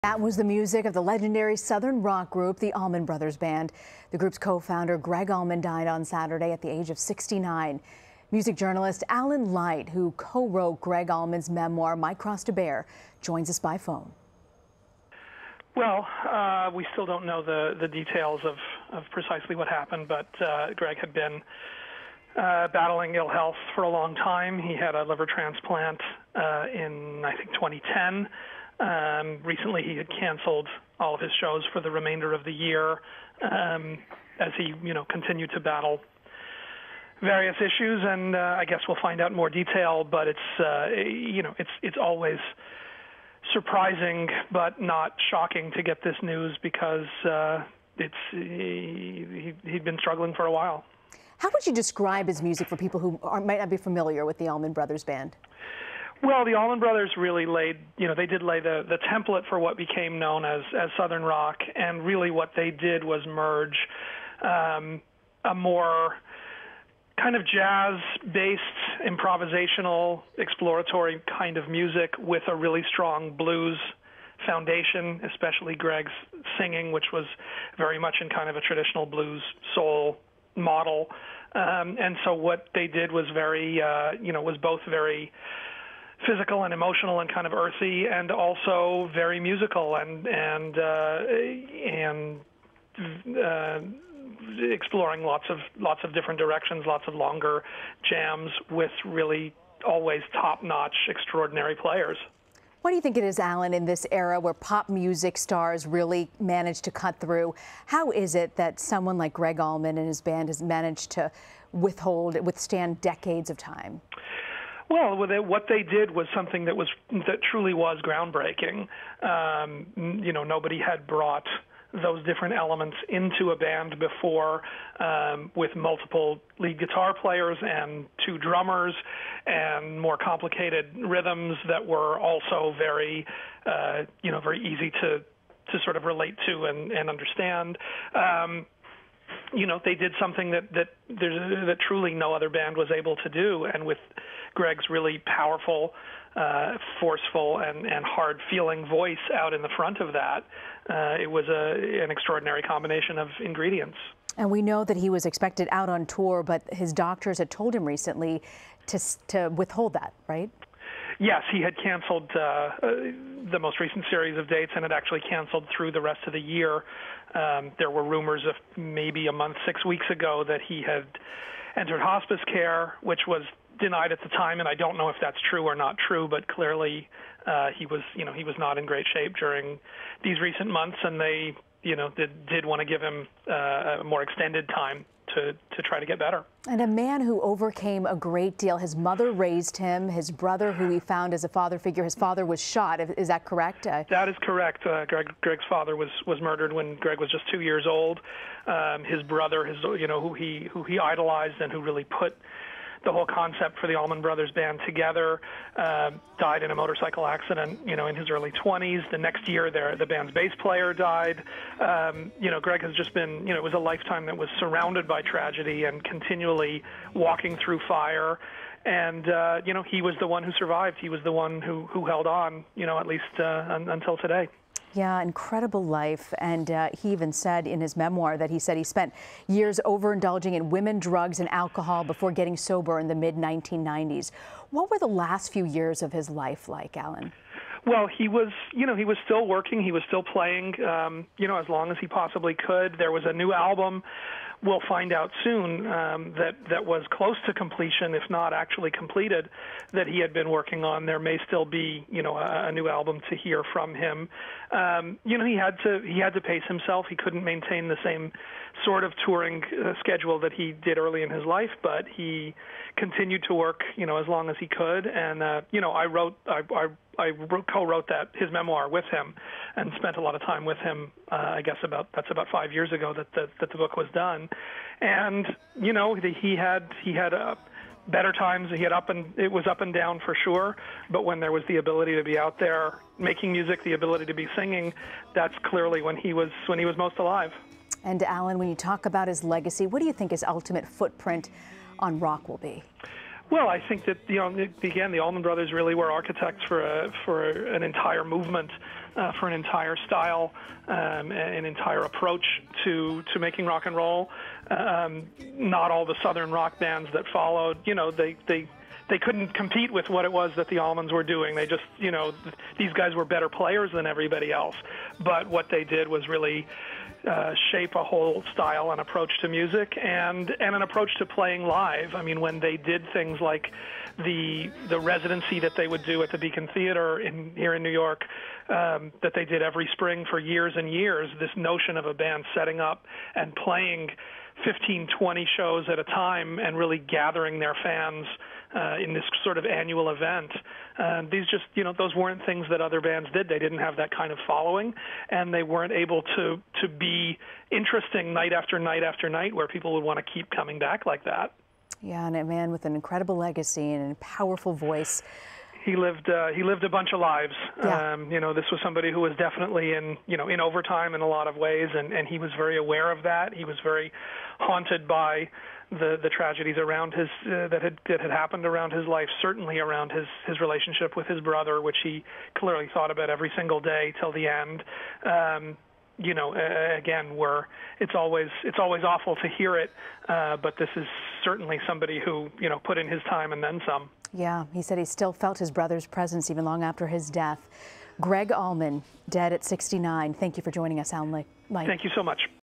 Gregg Allman, icon of Southern Rock, dead at 69
Gregg Allman, the soulful singer-songwriter who founded the Allman Brothers Band with his late brother, has died. He was 69. Allman was one of the pioneers of the Southern Rock movement.